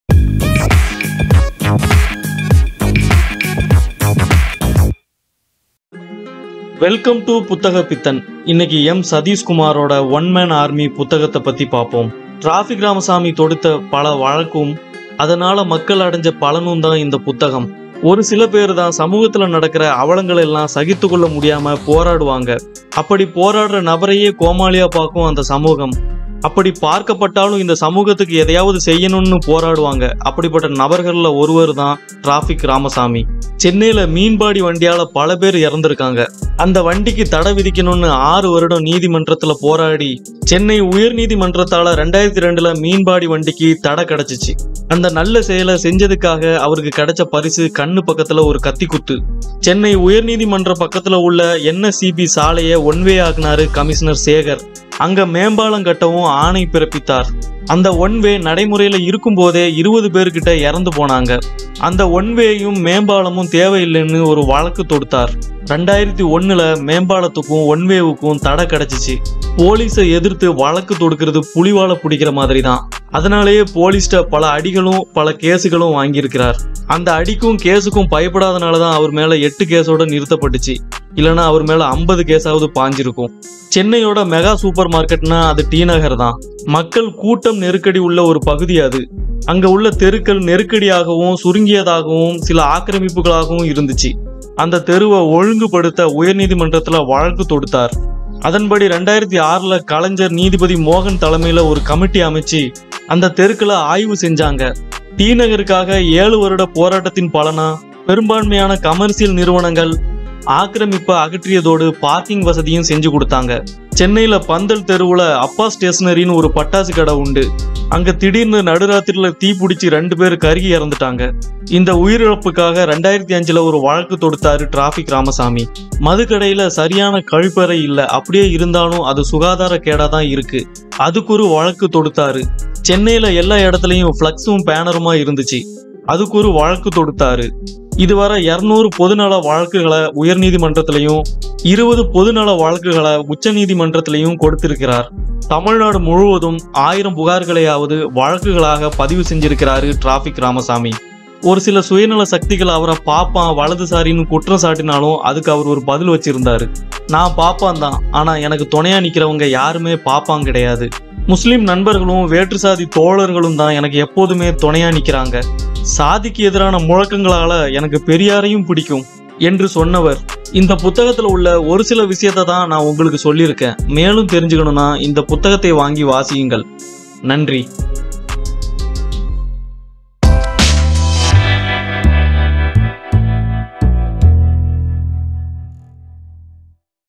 Puthaga Pithan | ஒன் மேன் ஆர்மி | எம் சதீஷ்குமார் | one man army | Traffic Ramasamy...
It is not only the achievers who teach life, but the common man's natural courage and inspiration to speak the truth. Whenever I am faced with the question of what a person can do, I think what is there that a person cannot do. Good inspirational narration for younger generation. Traffic Ramasamy was a true inspiration. He was the one man army.